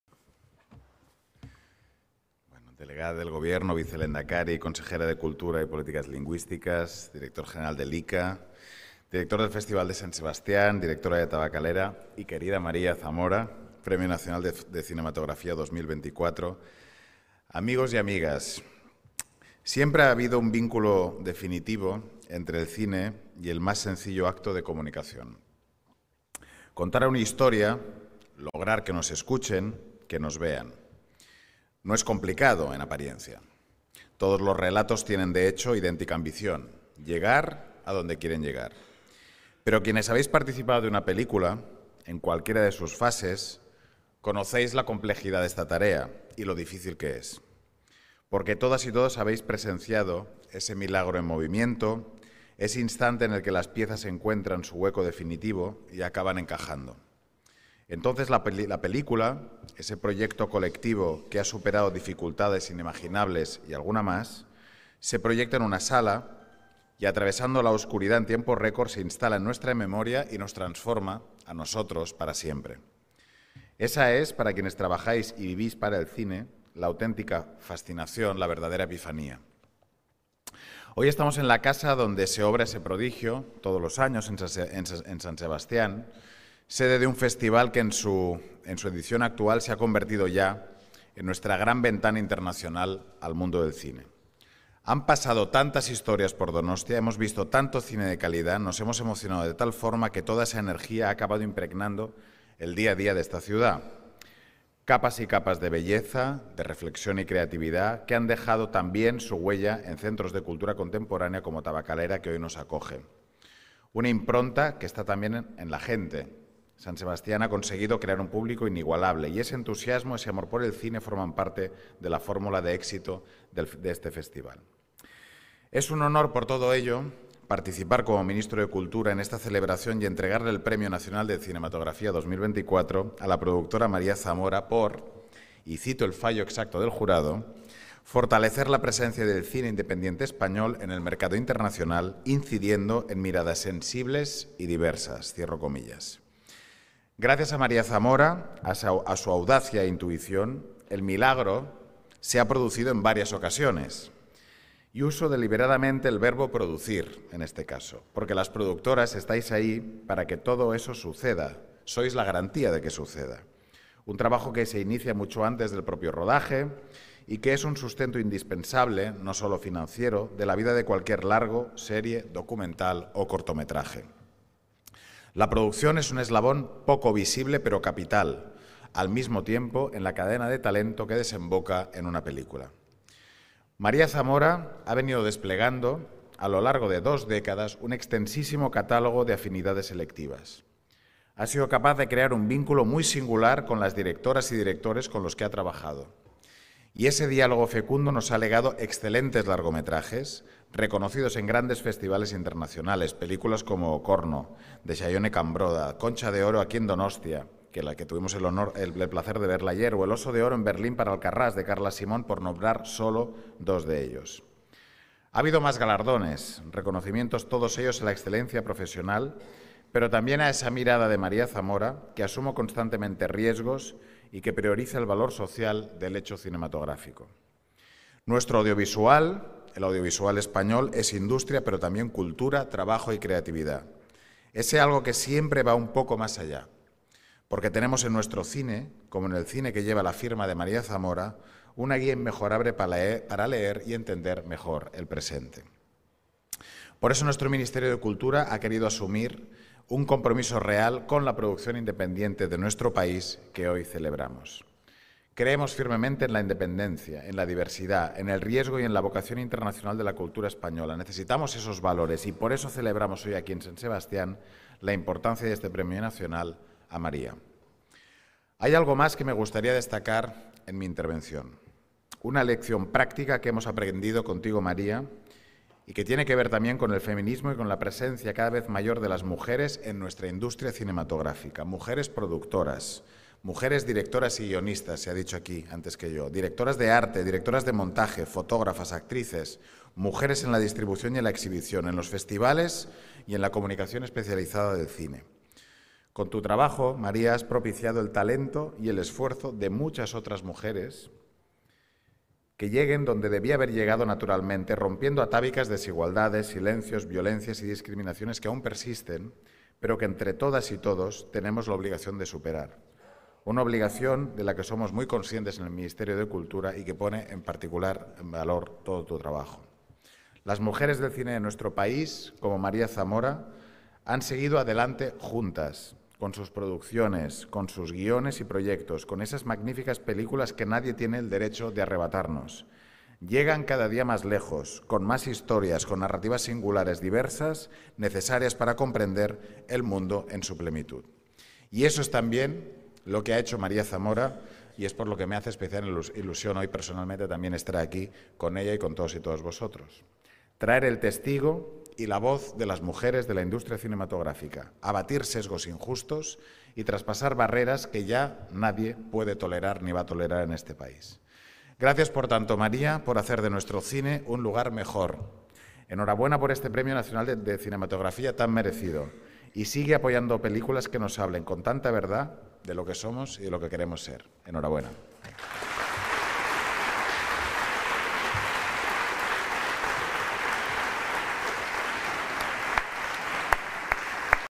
El ministro de Cultura, Ernest Urtasun, entrega a María Zamora el Premio Nacional de Cinematografía
El ministro de Cultura, Ernest Urtasun, ha entregado el Premio Nacional de Cinematografía 2024 a la productora de cine María Zamora, en el marco del Festival de Cine de San Sebastián.

Durante su discurso, Ernest Urtasun se ha dirigido a la galardonada para expresar que “con tu trabajo has propiciado que el talento y el esfuerzo de muchas otras mujeres llegue a donde debía haber llegado naturalmente, rompiendo atávicas desigualdades, silencios, violencias y discriminaciones que aún persisten, pero que entre todas y todos tenemos la obligación de superar. Una obligación de la que somos muy conscientes en el Ministerio de Cultura”.